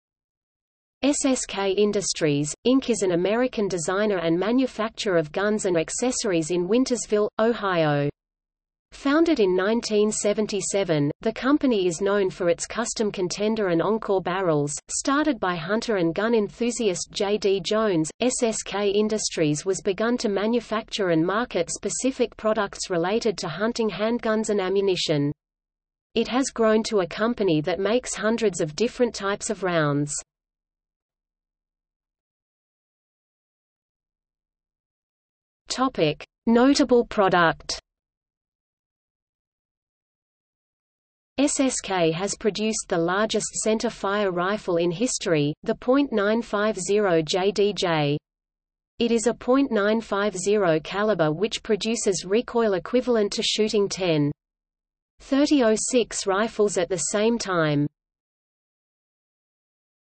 SSK Industries | Wikipedia audio article
00:00:32 Notable Product

- Socrates

SUMMARY
SSK Industries, Inc. is an American designer and manufacture of guns and accessories in Wintersville, Ohio. Founded in 1977, the company is known for its custom Contender and Encore barrels.Started by hunter and gun enthusiast J. D. Jones, SSK Industries was begun to manufacture and market specific products related to hunting handguns and ammunition. It has grown to a company that makes hundreds of different types of rounds.